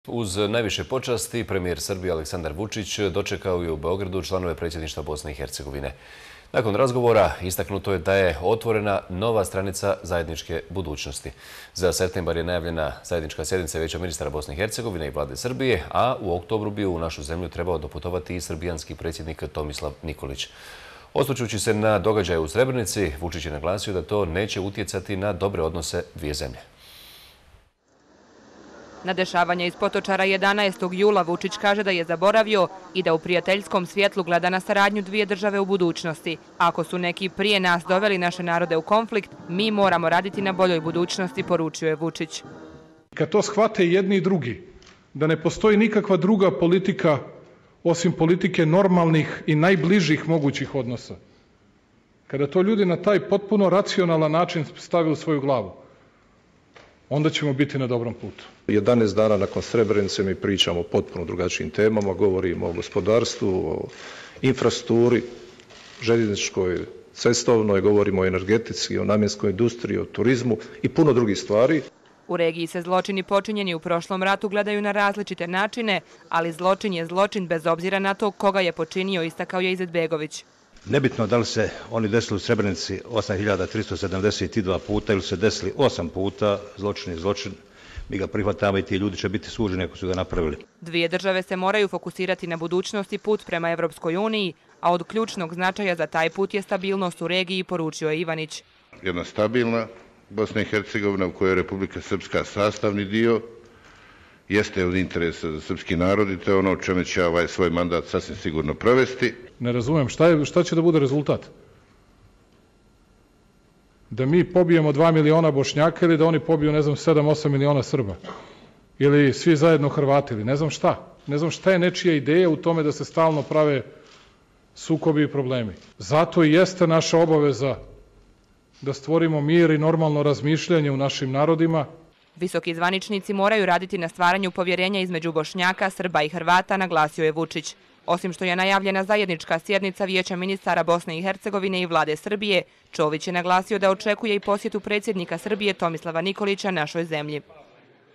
Članovi Predsjedništva BiH i premijer Srbije saglasni, saradnja i stabilnost garant bolje budućnosti
Zajednička sjednica Vijeća ministara BiH i Vlade Srbije, bit će održana u septembru, najavio je predsjednik Vlade Srbije, Aleksandar Vučić, nakon sastanka sa članovima Predsjedništva BiH.

Osvrčući se na događaje od 11. jula u Srebrenici, Vučić je naglasio da to neće uticati na dobre odnose dviju zemalja.

Na dešavanja iz Potočara 11. jula, Vučić kaže da je zaboravio i da u prijateljskom svjetlu gleda na saradnju dvije države u budućnosti. Ako su neki prije nas doveli naše narode u konflikt, mi moramo raditi na boljoj budućnosti, poručio je Vučić.

“I kad to shvate i jedni i drugi da ne postoji nijedna druga politika, osim politike najbližih i normalnih odnosa, kada to ljudi na potpuno racionalan način stave u svoju glavu onda ćemo biti na pravom putu“, istakao je premijer Srbije Aleksandar Vučić.

„11 dana nakon Srebrenice mi pričamo o potpuno drugačijim temama, govorimo o gospodarstvu, infrastrukturi, željezničkoj, cestovnoj, govorimo o energetici, namjenskoj industriji, turizmu i puno drugih stvari“, poručio je predsjedavajući Predsjedništva BiH Dragan Čović.

U regiji se zločini počinjeni u prošlom ratu gledaju na različite načine, ali zločin je zločin, bez obzira na to ko ga je počinio, istakao je član Predsjedništva BiH Bakir Izetbegović.

„Nebitno da li su se oni desili u Srebrenici 8.372 puta ili su se desili osam puta, zločin je zločin, mi ga prihvatamo i ti ljudi će biti suđeni ako su ga napravili.“

Dvije države se moraju fokusirati na budućnost i put prema EU, a od ključnog značaja za taj put je stabilnost u regiji, poručio je član Predsjedništva BiH Mladen Ivanić.

„Jedna stabilna BiH, u kojoj je RS sastavni dio, jeste od interesa za srpski narod i to je ono u čemu ću ja ovaj svoj mandat sasvim sigurno provesti.“

„Ne razumijem šta će da bude rezultat? Da mi pobijemo dva miliona Bošnjaka ili da oni pobiju sedam miliona Srba? Ili svi skupa Hrvate ili ne znam šta? Šta je nečija ideja u tome da se stalno prave sukobi i problemi? Zato i jeste naša obaveza da stvorimo mir i normalno razmišljanje u našim narodima“, tvrdi Vučić.

Visoki zvaničnici moraju raditi na stvaranju povjerenja između Bošnjaka, Srba i Hrvata, naglasio je Vučić. Osim što je najavljena zajednička sjednica Vijeća ministara BiH i Vlade Srbije, Čović je naglasio da očekuje i posjetu predsjednika Srbije Tomislava Nikolića našoj zemlji.